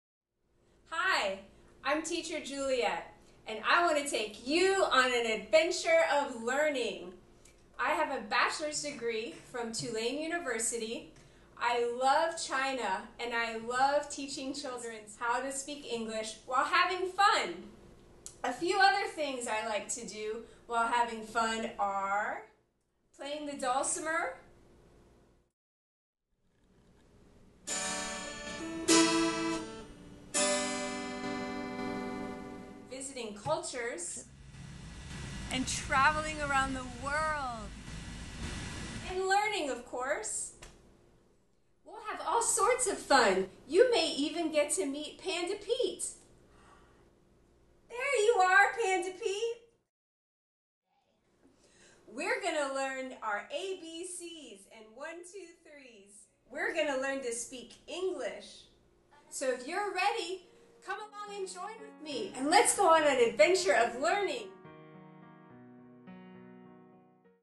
Teacher Juliette
This is a video I made for my friend Juliette to get chosen to teach children in China how to speak English online.